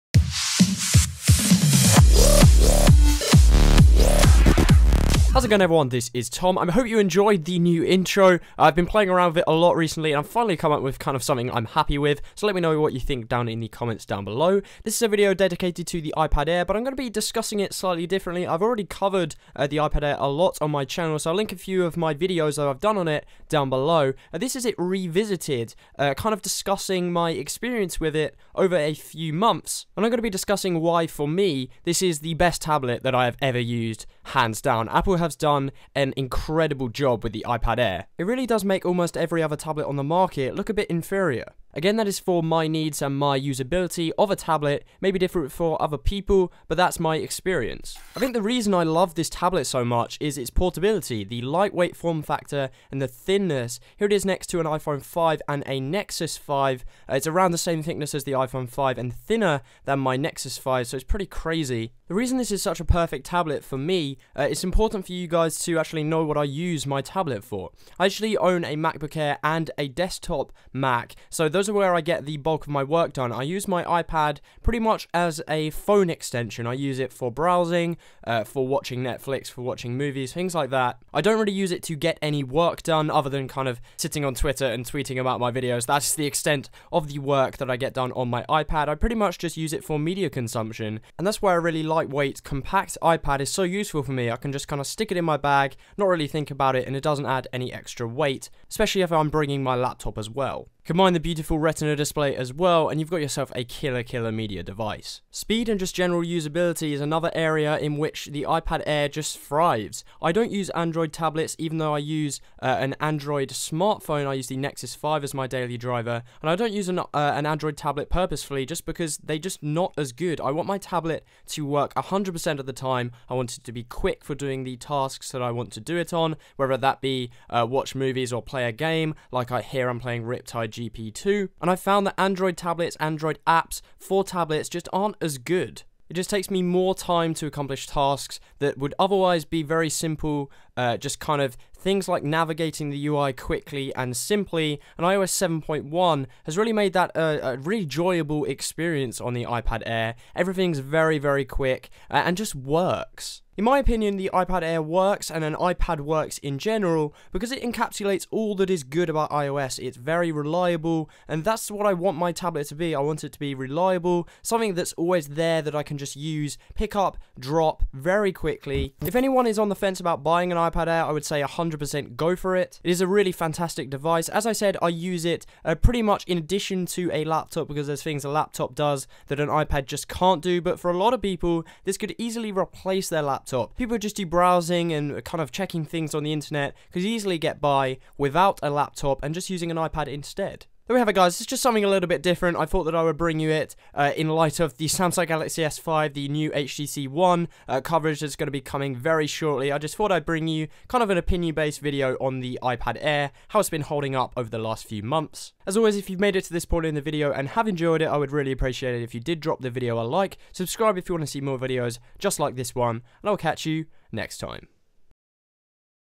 Apple iPad Air: Revisited!
How is Apple's newest tablet performing after nearly 6 months? Let's find out.

More Info!

FAQs!

My watch (US)
My watch (UK)